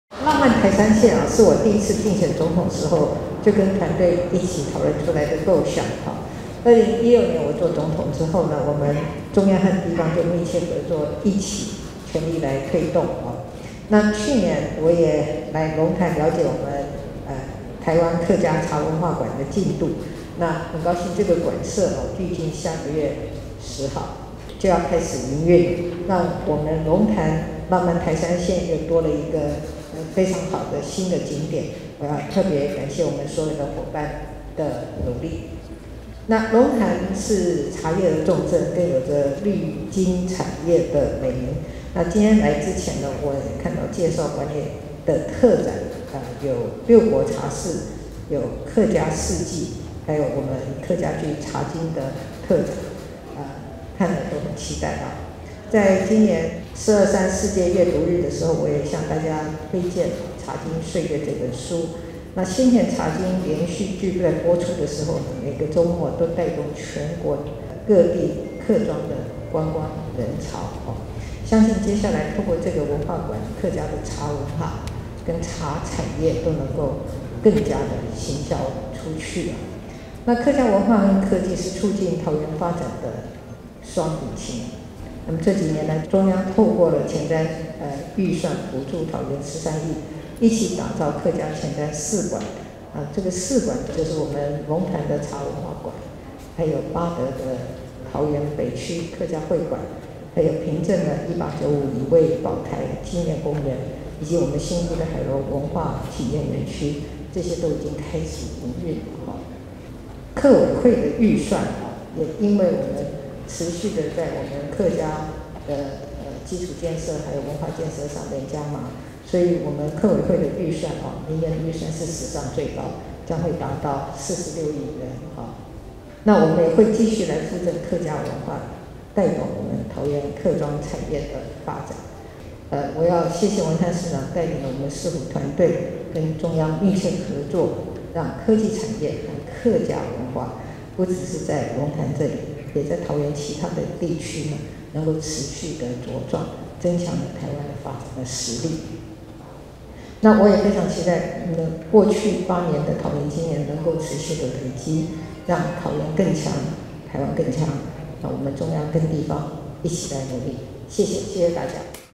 視察台灣客家茶文化館 蔡英文：復振客家文化 帶動桃園客庄產業發展
蔡英文總統今天（11/17）下午前往桃園視察「台灣客家茶文化館」時表示，這幾年來，中央透過前瞻預算補助桃園13億元，一起打造客家前瞻四館。除了茶文化館將於下個月10號開始營運，桃園北區客家會館、1895乙未保台紀念公園及海螺文化體驗園區都已開始營運。明年，客委會的預算將達到46億元，政府會復振客家文化，帶動桃園客庄產業的發展。

蔡英文致詞時表示，「浪漫台三線」是她第一次競選總統時跟團隊一起討論的構想；2016年，她擔任總統以後，中央與地方密切合作，一起全力推動；去年，她也來到龍潭，瞭解「台灣客家茶文化館」的進度。很高興這個館舍，預計在下個月10號即將開始營運，讓這裡又多了一個新景點，她要特別感謝所有夥伴的努力。

蔡英文指出，龍潭是茶葉重鎮，更有著「綠金產業」的美名。「台灣客家茶文化館」館內的特展，有六國茶室、客家四季，還有客家劇《茶金》的特展，令人非常期待。

蔡英文說，在今年423世界閱讀日，她也向大家推薦《茶金歲月》這本書。先前《茶金》連續劇播出之後，每個週末都帶動全國各地客庄的觀光人潮。相信未來，透過這個文化館，客家的茶文化、茶產業，能夠更加行銷出去。

蔡英文認為，客家文化和科技是促進桃園發展的雙引擎。這幾年來，中央透過前瞻預算補助桃園13億元，一起打造客家前瞻四館，除了龍潭的茶文化館，八德的桃園北區客家會館、平鎮的1895乙未保台紀念公園、以及新屋的海螺文化體驗園區，都已經開始營運。因為我們持續在客家基礎建設及文化建設上加碼，客委會明年的預算將是史上最高，達到46億元。我們會繼續復振客家文化，帶動桃園客庄產業的發展。

蔡英文說，不僅振興文化，桃園市長鄭文燦更看到桃園的未來。這幾年來，鄭文燦市長努力發展半導體聚落。當台積電規劃要在臺灣擴大投資先進製程，鄭文燦市長、鄭運鵬委員就注意到這個機會對桃園未來的重要性，一定要為桃園爭取並創造更好的條件。經過他們兩位與中央部會的共同努力，現在，龍潭科學園區三期擴建計畫已經正式啟動。這也代表，未來桃園會是全球半導體最先進製程的首選。

蔡英文表示，不論是半導體產業，還是客家文化，都是台灣在國際上的優勢和特色。她要感謝鄭文燦市長帶領市府團隊，跟中央密切合作，讓科技產業和客家文化，在龍潭與桃園其他的地區持續茁壯，增強台灣的發展實力。她期盼，過去8年的桃園經驗能夠持續累積，讓桃園更強、台灣更強，也盼中央與地方，一起持續努力。